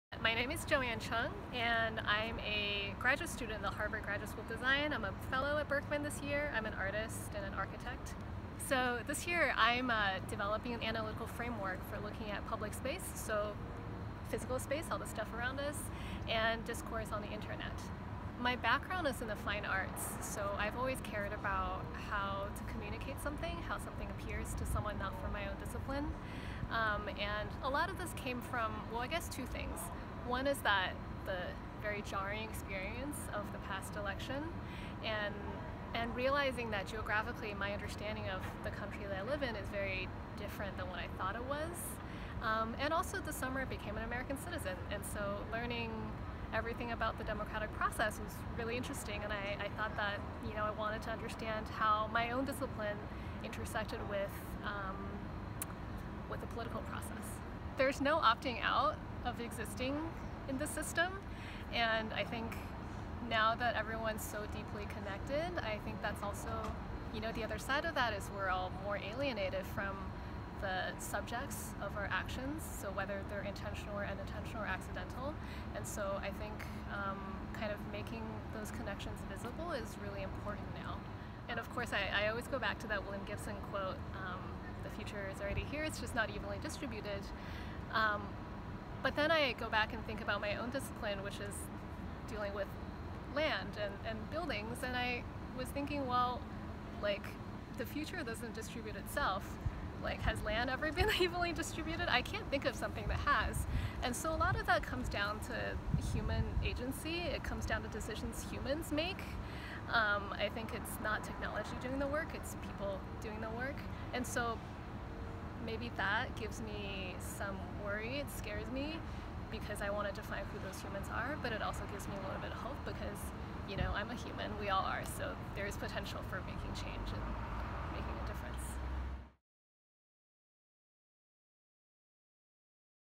Fellow Friday: Joanne Cheung
Learn about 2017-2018 BKC fellow Joanne Cheung, who examines public space and discourse on the Internet.